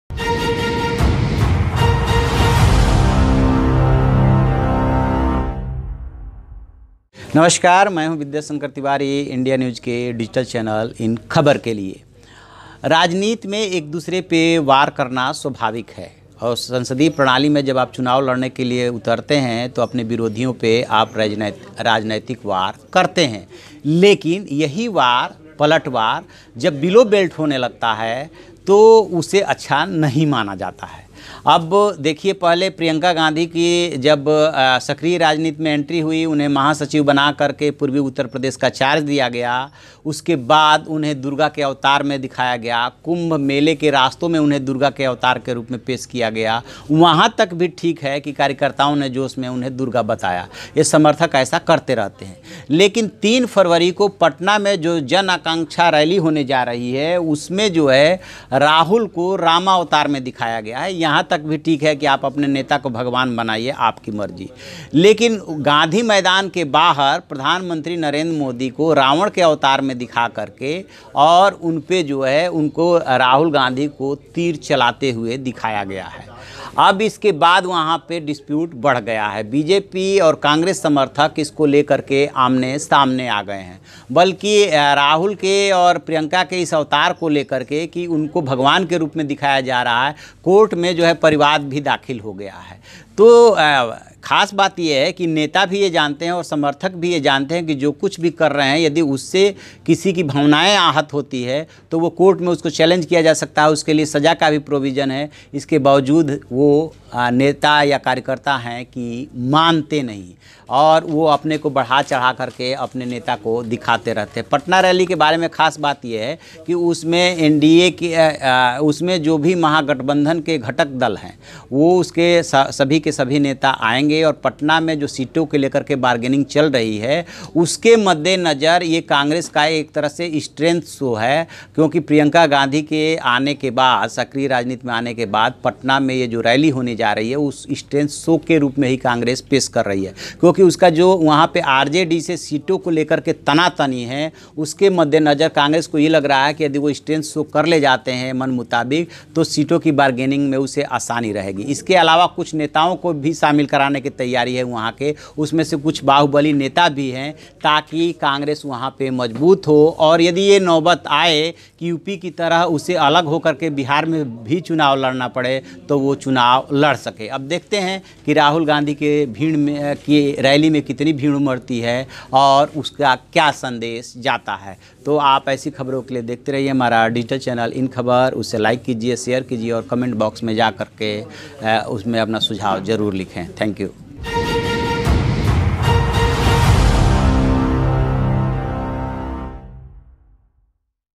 पटना जन आकांक्षा रैली में PM Modi को Congress ने पोस्टर में बनाया ‘रावण’, राहुल गांधी बने भगवान राम
पटनाः लोकसभा चुनाव की घोषणा भले ही अभी नहीं हुई हो. लेकिन भाजपा और कांग्रेस अभी से आमने सामने हैं. कांग्रेस अपनी जन आकांक्षा रैली के बहाने बीजेपी और प्रधानमंत्री नरेंद्र मोदी पर लगातार पोस्टर वॉर कर रही है. कांग्रेस द्वारा एक बार फिर राहुल गांधी को भगवान राम और पीएम नरेंद्र मोदी के दुशासन वाला पोस्टर लगाए जाने के बाद बिहार की राजनीति गरमा गई है.